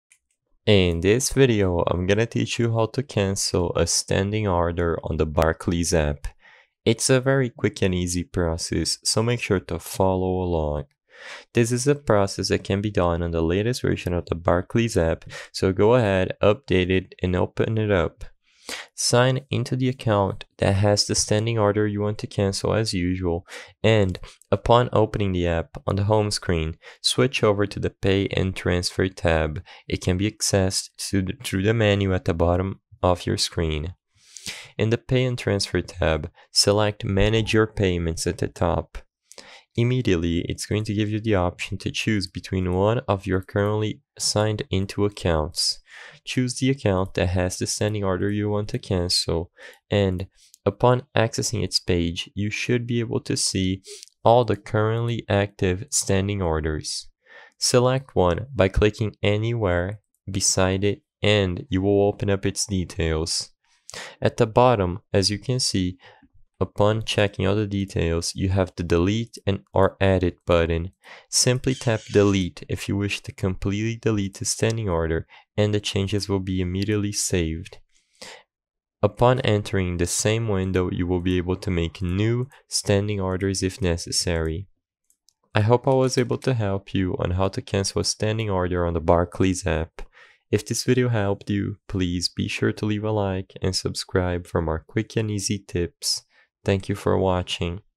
How To Cancel Standing Order on Barclays App (EASY!)
In this video I will show you how to cancel standing order on barclays app.

I hope I have helped you with the video!